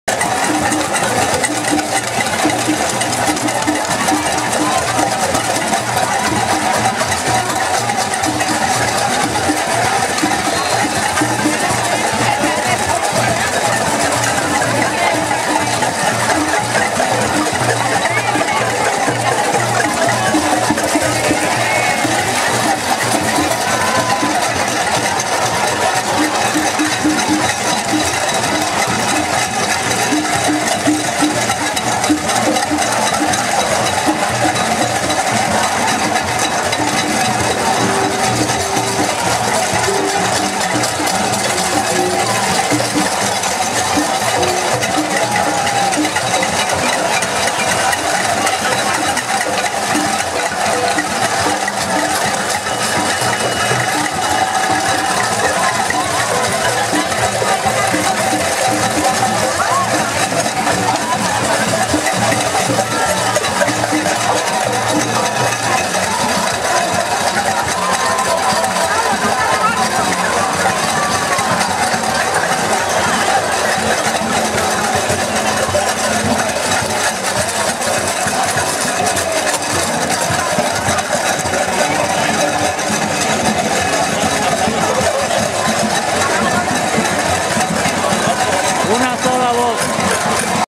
MARCHA UNA SOLA VOZ UNIDOS POR EL PUEBLO NESECITADO ''
MAS DE CIEN ORGANIZACIONES SE UNEN PARA MANIFESTARCE EN CONTRA DE LOS RECORTES DE FONDOS DEL GOBIERNO DE PUERTO RICO. A TODA ORGANIZACION E INSTITUCION SIN FINES DE LUCRO QUE SIRVEN AL PAIS EN MEJOR FORMA QUE EL MISMO GOBIERNO .